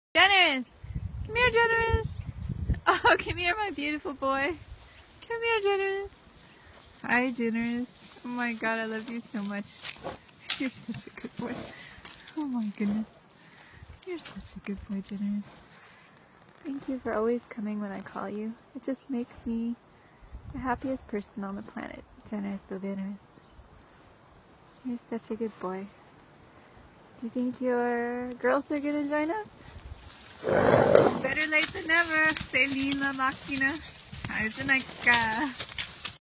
Generous in July 2016
He always comes galloping when called! LOVE that.